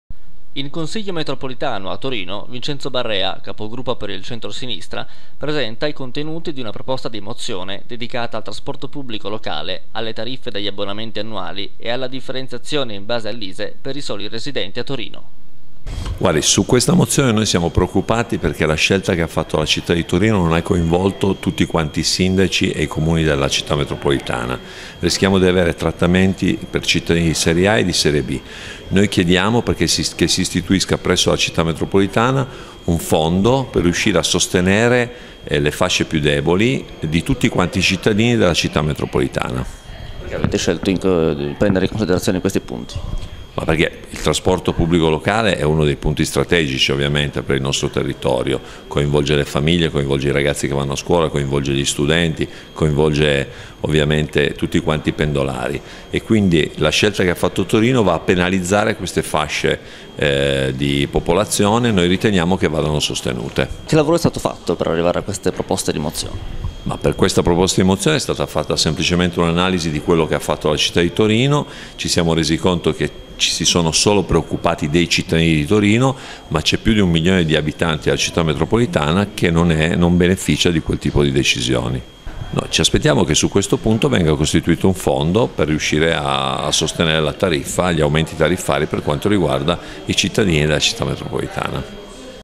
TORINO: Mozione dedicata al trasporto pubblico - 27.07.2018 GRP Tv
"Chiediamo che si istituisca, presso la Città Metropolitana, un fondo per riuscire a sostenere le fasce più deboli di tutti quanti i cittadini della città metropolitana". In Consiglio Metropolitano a Torino Vincenzo Barrea, capogruppo per il Centrosinistra, ha presentato una proposta di mozione dedicata al trasporto pubblico locale, alle tariffe degli abbonamenti annuali e alla differenziazione in base all'Isee per i soli residenti a Torino.